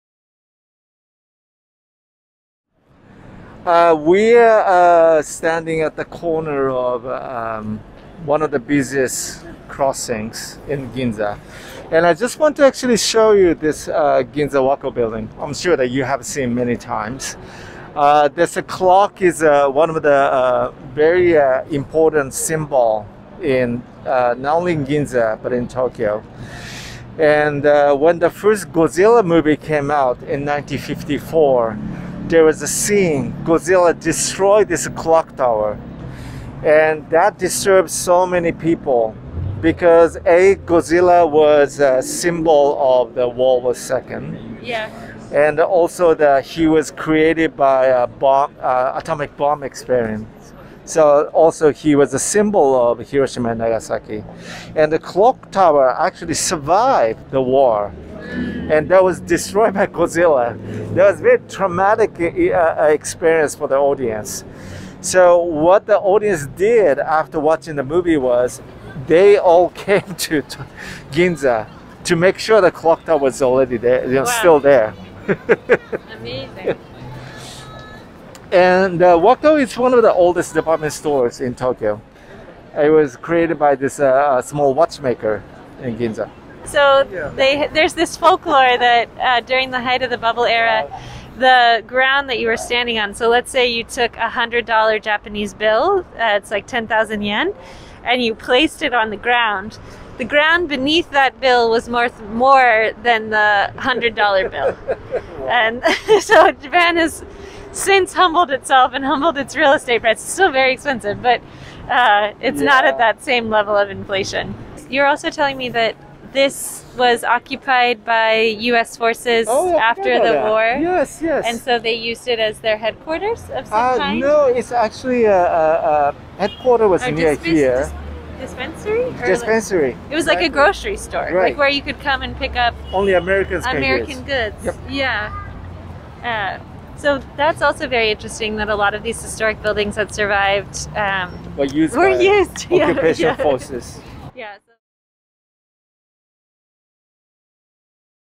Stop 9: Wako Building
Like Hermès, Wako started out as a watchmaker and over the years grew into one of Japan's most luxurious department stores. The building that we see today survived the bombings of WW2 and was used as a commissary by the occupational forces. However, the 1954 film, Godzilla showed the clock tower being destroyed, a hugely traumatic event for Tokyoites who flocked to the intersection to make sure the clock was still standing.